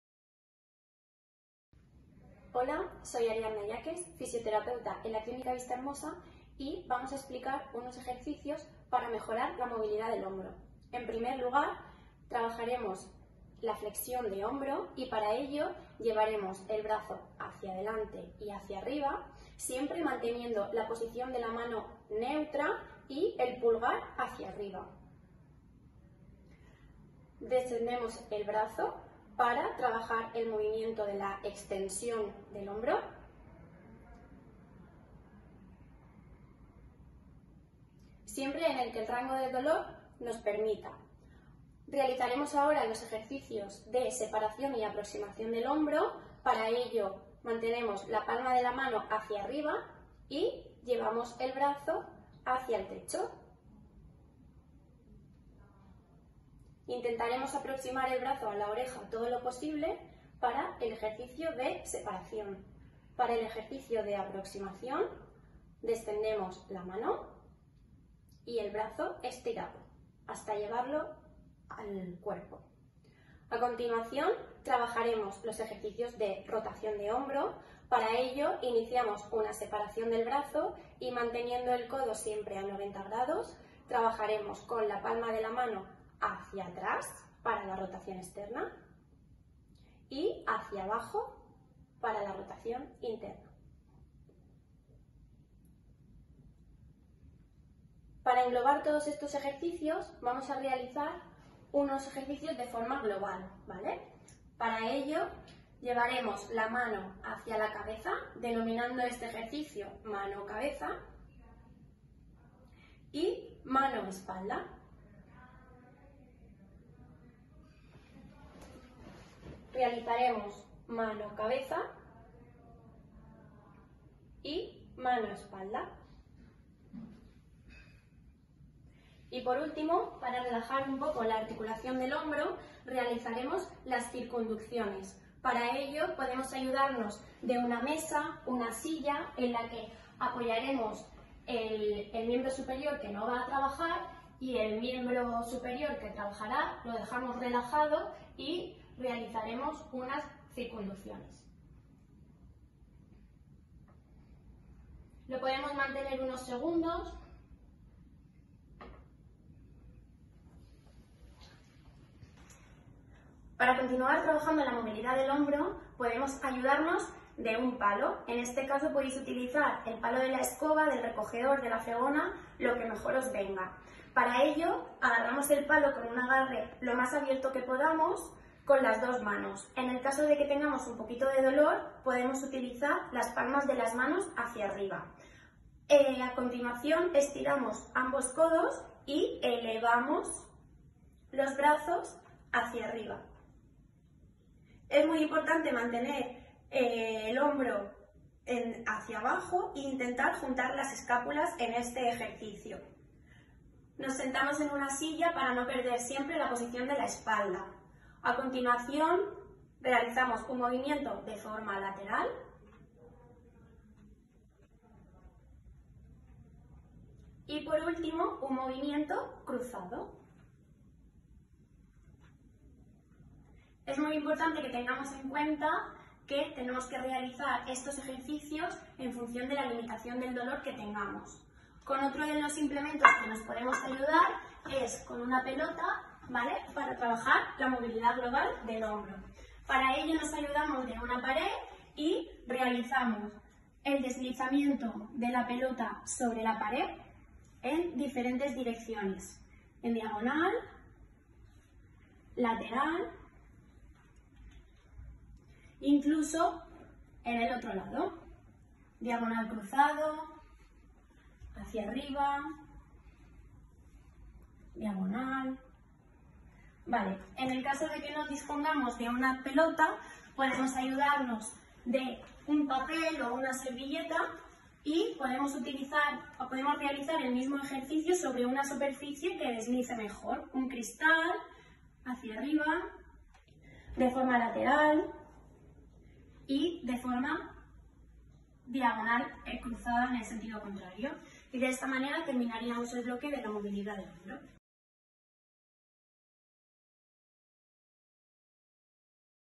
HOMBRO: MOVILIDAD.
El equipo de fisioterapia nos enseña una serie de sencillos ejercicios para mejorar la movilidad del hombro.